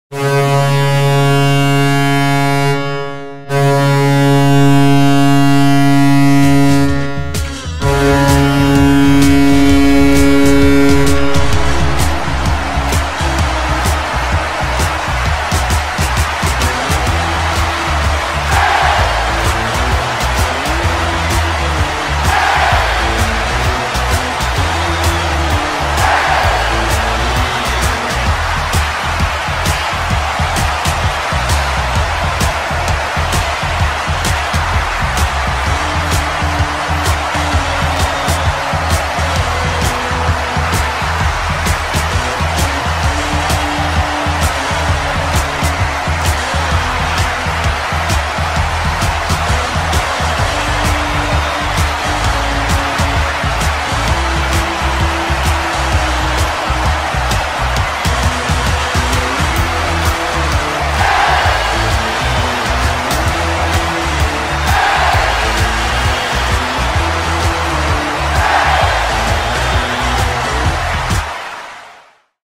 Colorado Eagles 2022 Goal Horn
Colorado Eagles goal horn used during the 2021-22 AHL season at Budweiser Events Center in Loveland, CO.